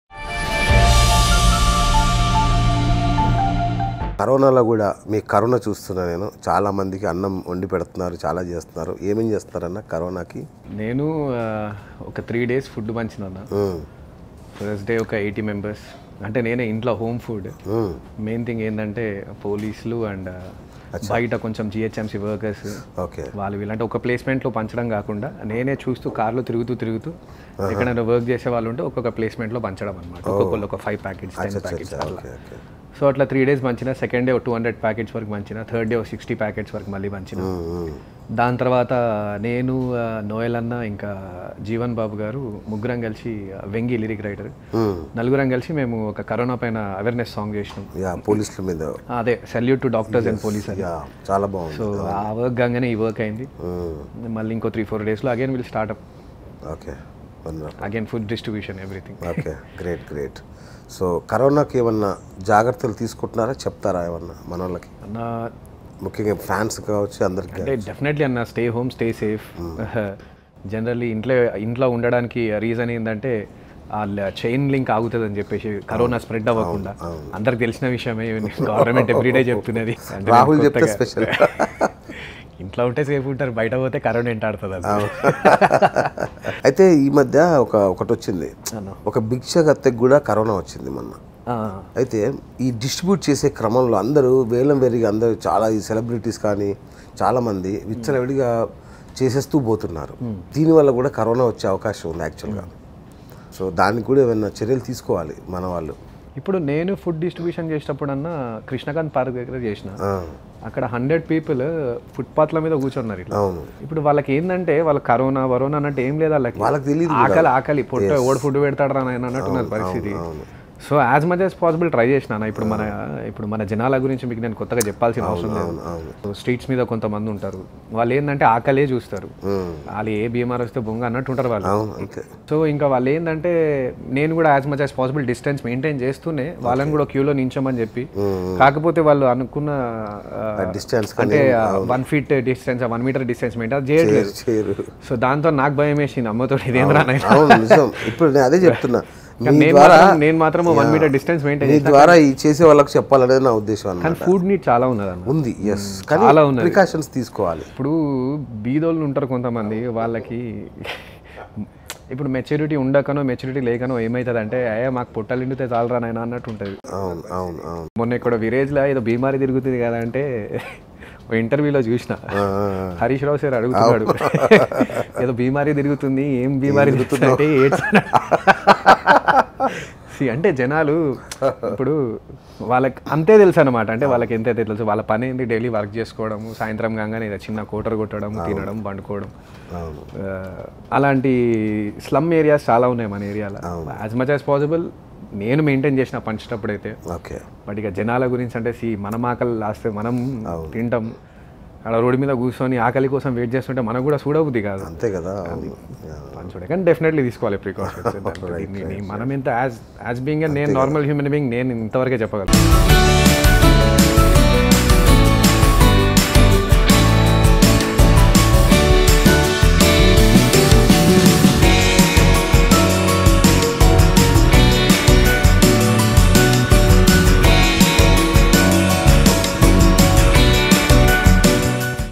Singer Rahul Sipligunj Expalined Harish Rao Funny Moment | Telangana TV
Singer Rahul Sipligunj Expalined Harish Rao Funny Moment